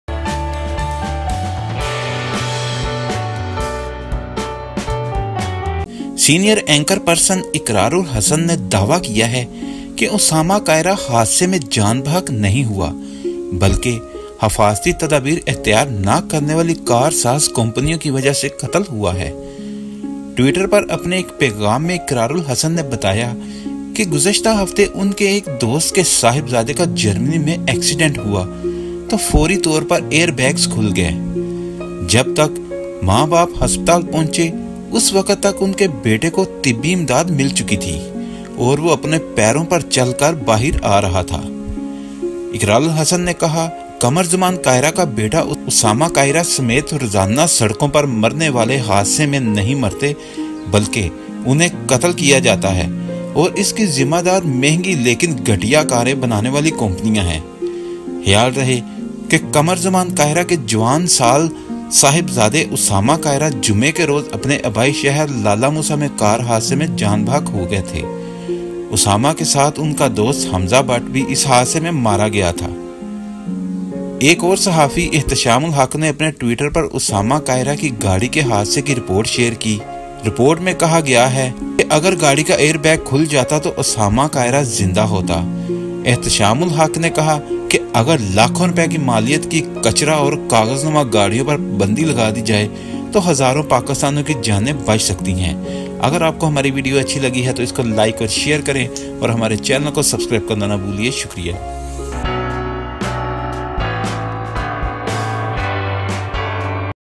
Ab Tak Ki Sab Se heran Kun Khabar.
Until the parents reached the hospital, his son's medical aid was over and he came out on his feet. Khalid Zaman Kaira's son did not die in the accident that died on daily roads including Osama KARACHI. But they are killed and they are expensive, but expensive cars are companies. It seems to be that the friend of Qamar Zaman Kaira, who was killed in a car accident in Osama Kaira on Friday, was killed in his native town Lalaa Mosa. Osama, along with his friend, Hamza Butt, was killed. Another journalist, Akhtaram ul Haq, shared the report of Osama KARACHI's car crash on Twitter. The report said that Osama Kira would have been alive if the vehicle's air bag was opened. Hukumam ul Haq said that thousands of Pakistaniis can be saved if millions of rupees worth the ban and paperwork vehicles are banned.